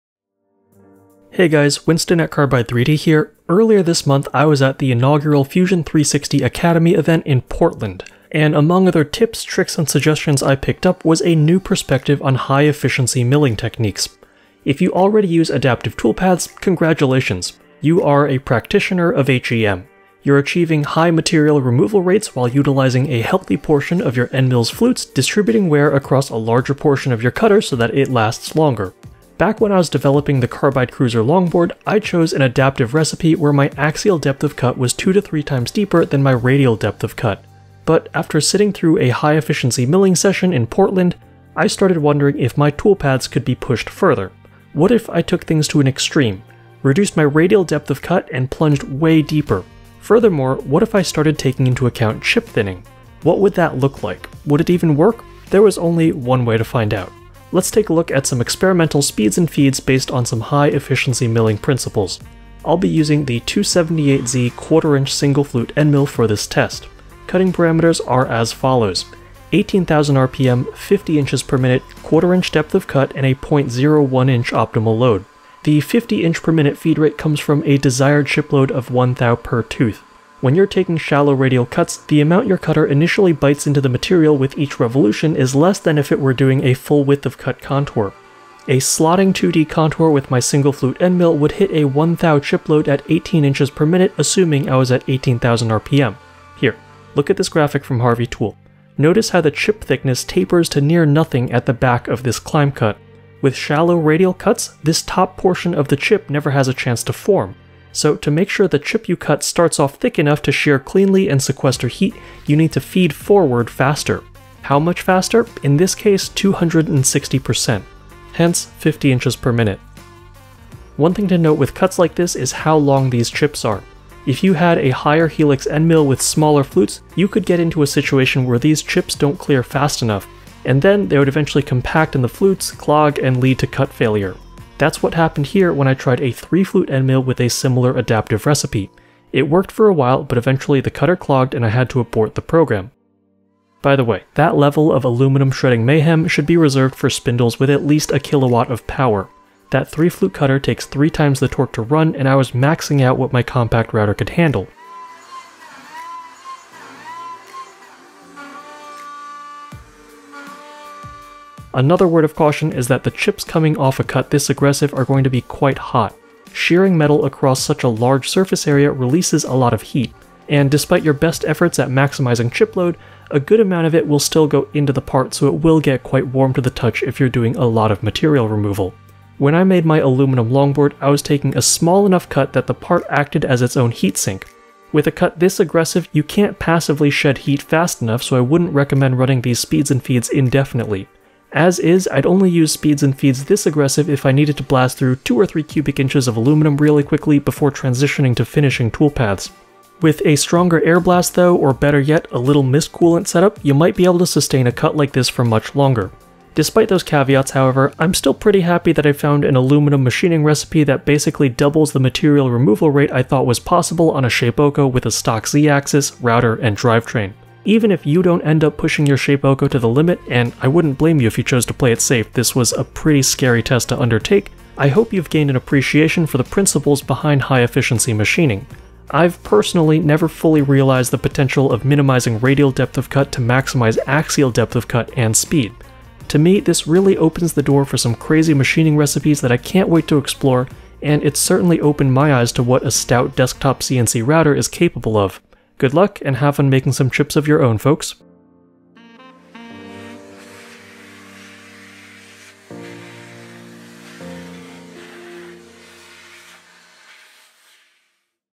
Testing High Efficiency Milling Techniques on a Shapeoko 3
If you already use adaptive toolpaths, congratulations. You are a practitioner of HEM. You’re achieving high material removal rates while utilizing a healthy portion of your endmill’s flutes, distributing wear across a large portion of your endmill’s flutes so it lasts longer. But what if we take that concept to the extreme...?

Pro-tip: Keep a few thou more stock to leave than usual because there is more deflection (axial and radial) when the spindle is so heavily loaded. I use about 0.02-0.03" stock to leave when roughing, HDZ users may need less.

Learn more about our lines of Desktop CNC machines at carbide3d.com